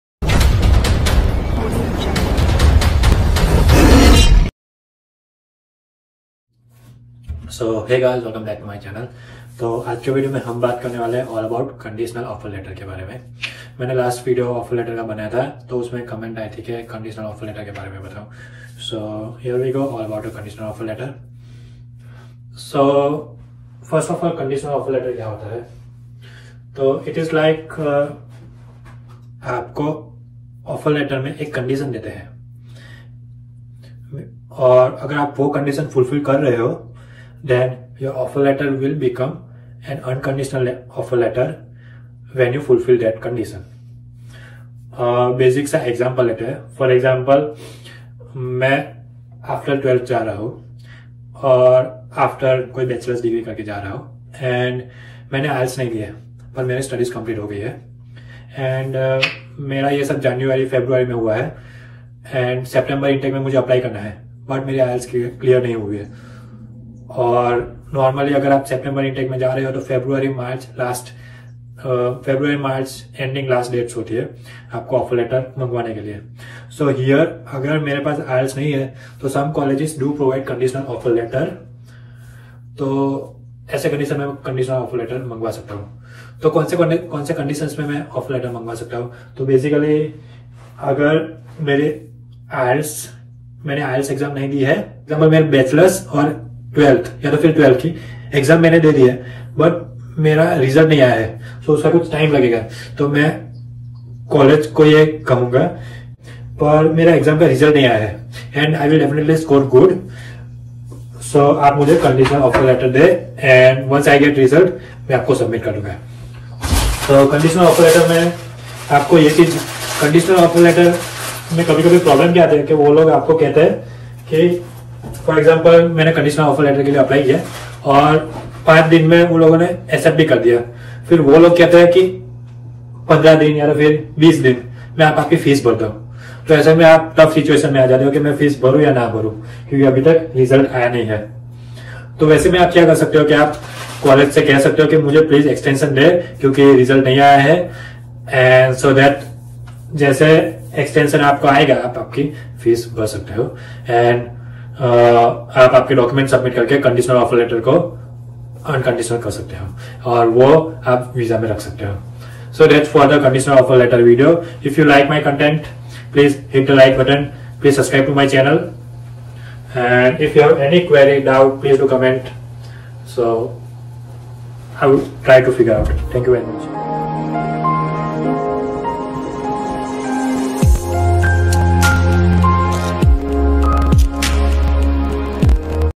What is Conditional Offer letter? Study in Canada Step 1
Hello viewers, '
In this video, I have discussed all regarding the conditional offer letter for students pursuing dream for Canada.